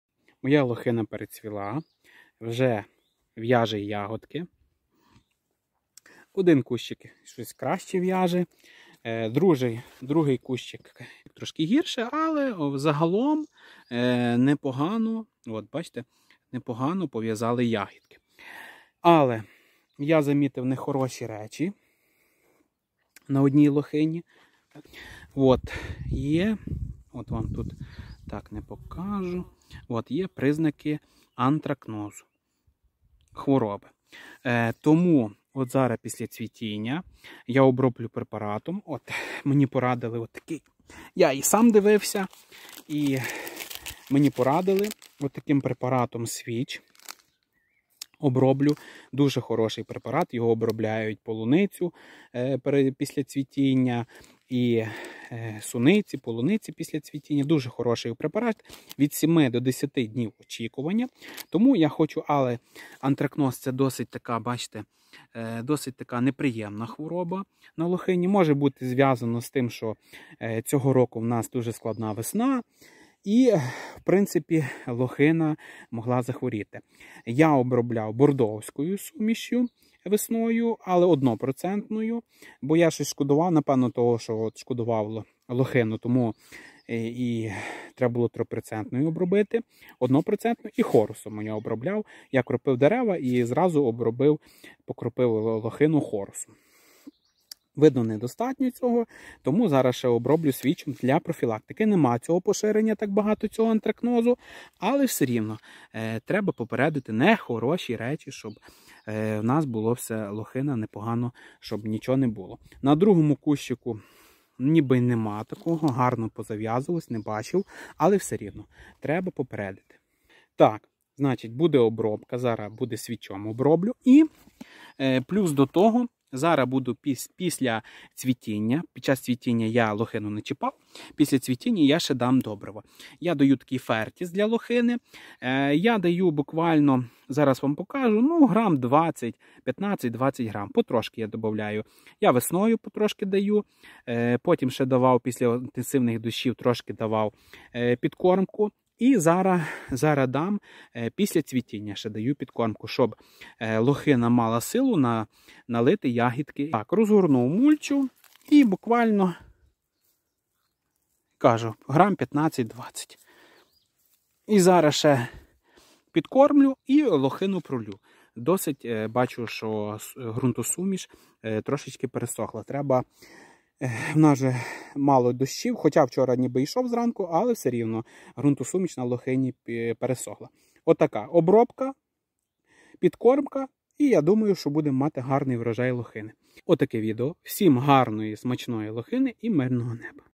Підкормка та захист Лохини після цвітіння
Лохина на дачі вже перецвіла, тому робимо підкормку макро і мікроелементами для гарного налиття ягід і обробляємо лохину від хвороби, бо є признаки такої хвороби як Антракноз.

Стосовно вирощування лохини в себе на дачі, то хочу поділитися своїм досвідом. Лохина дуже незвична, корисна і смачна ягода. Коли перший раз спробував - був вражений її неймовірним смаком. Всім рекомендую посадити лохину в себе на ділянці, якщо дотримуватися технології і доглядати за лохиною, ви точно не пожалієте.
Розповім певні нюанси щодо вирощування лохини на ділянці:

Перше - необхідно торф, бажано кислий і не вірте шарлатанам на базарі, які будуть вам розказувати, що можна садити в простий грунт. Їм просто треба продати вам саджанець лохини і заробити на вас гроші, а потім: чи він буде рости чи не буде - це вже не їх проблеми,  а ваша. І якщо вона у вас не виросте, пропаде чи не буде давати ягоди - вони скажуть, що ви й самі винні.

Друге - потрібне закислення торф'яної суміші, а саме додавати сірку до суміші і час від часу, якщо листя починає червоніти, я поливаю водою з оцетом. Даю 100 гр оцету на 1 відро і ділю на два кущі (перед цим пролийте простою водою), звичайно орієнтуйтесь, які у вас кущі.

Третє - якщо у вас важкий грунт (суглинистий) і часто йдуть дощі вам необхідно підняти рівень посадки десь на 20 см над грунтом, бо якщо буде стояти вода, то лохина пропаде.

І четверте - я додаю до торф'яної суміші дуже трошки старого перегною,  але без фанатизму. Перегній робить грунтосуміш більш поживнішою, покращує розвиток мікрофолори. Я вже два роки додаю: лохина в мене гарно родить і розвивається,  але звичайно вирішувати вам, бо додавати перегній до лохини - не рекомендують.

Щодо весняного підживлення лохини - одне з найважливіших для гарного розвитку і рясного врожаю. Основне добриво, яке я спеціально купляю для підкормки кущів лохини - FERTIS. Це комплексне литовське добриво для лохини. В ньому є основні макро і мікролементи для гарних та смачних ягід, гарнорго розвитку кущів та стійкості лохини до захворювань.

Крім того, у  зв'язку із заморозками і холодною весною лохина може хлорозити, у зв'язку із поганим засвоєнням кориневою системою поживних речовин. Листя лохини може почервоніти, більше чи менше багато залежить від сорту, не потрібно кидатися і рятувати кущі лохини чи інтенсивно підкислювати, з теплом лохина відновить нормальний колір свого листя.

Гарних та рясних врожаїв вам Лохини!